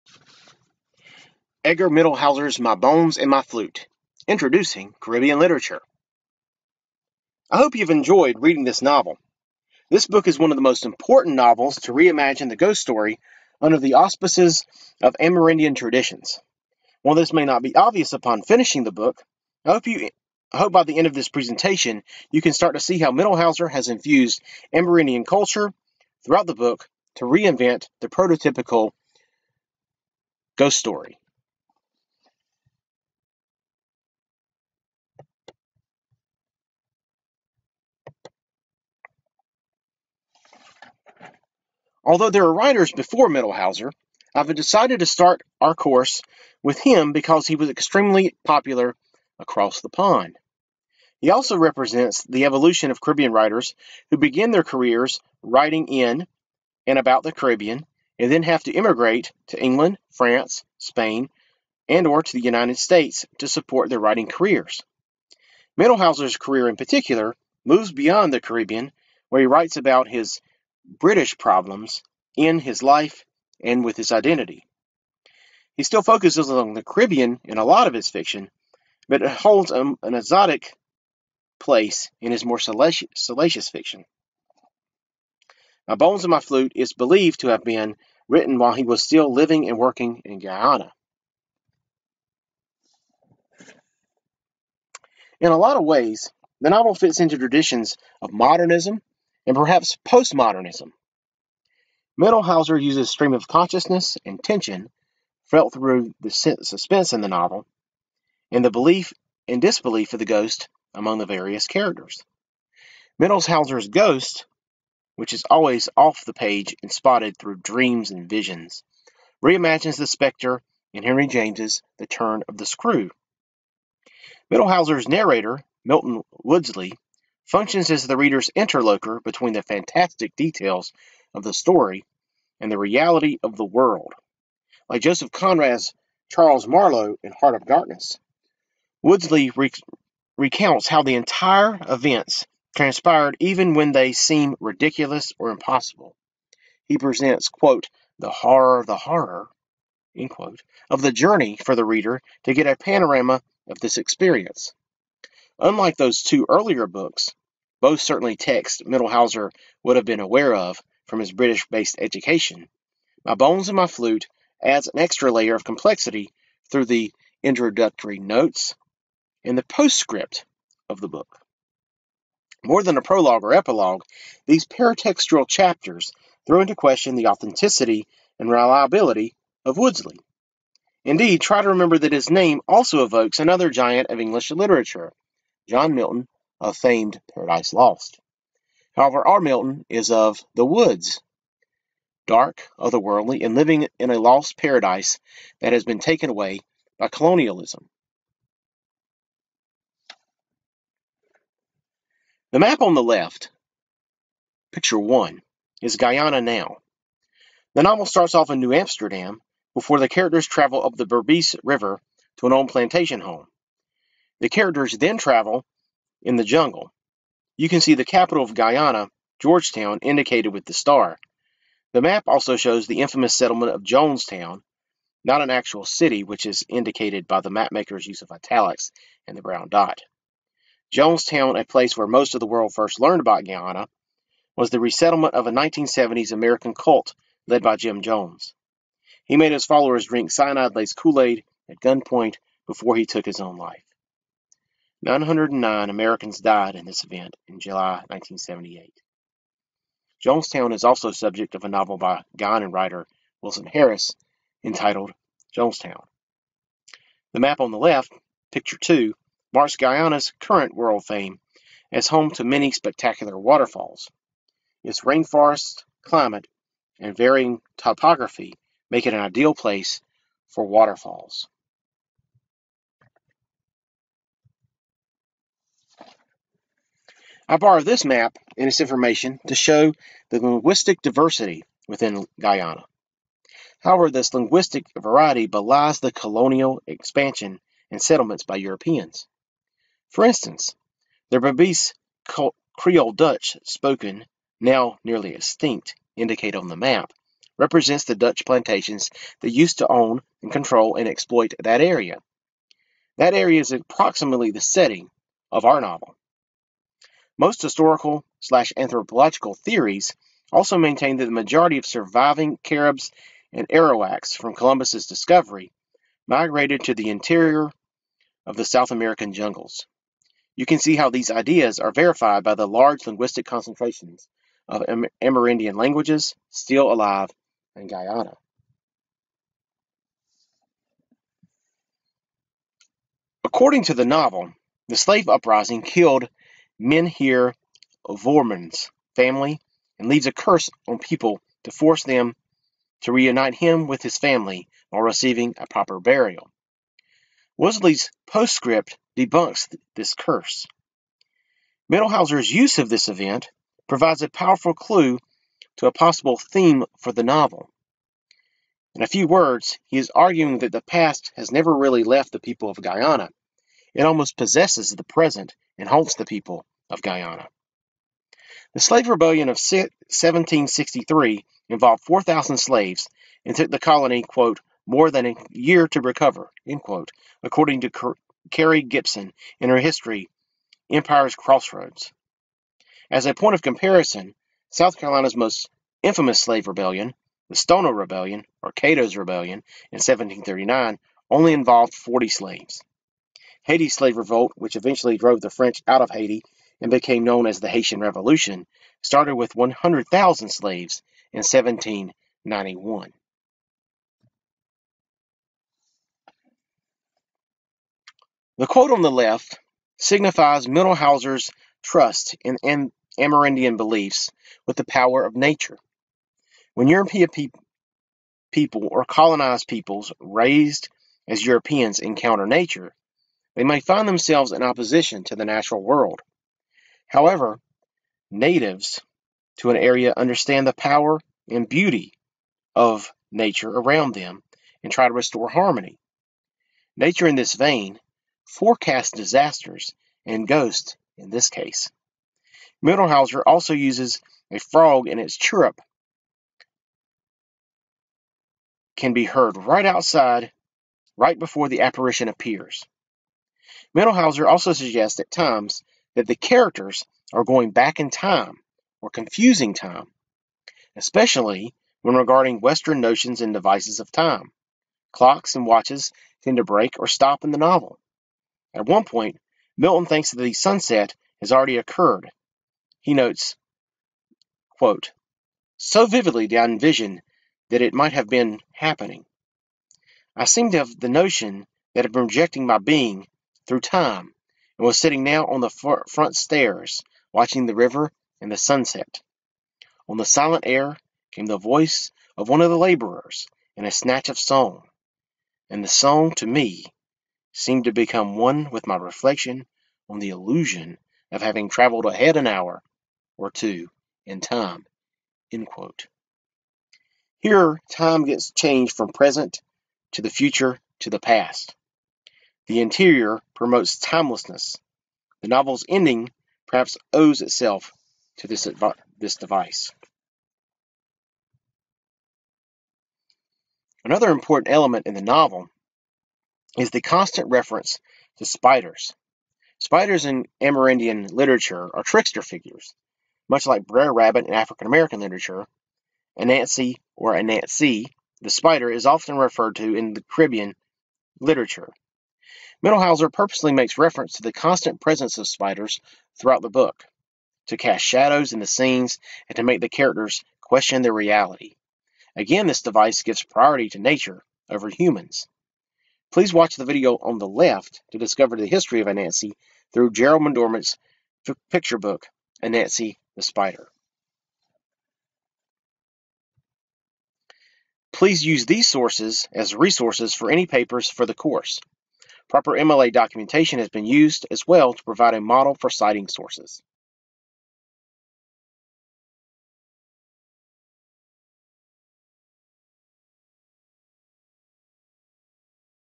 Edgar Mittelholzer's My Bones and My Flute
A brief exploration of the Guyanese novel My Bones and My Flute by Edgar Mittelholzer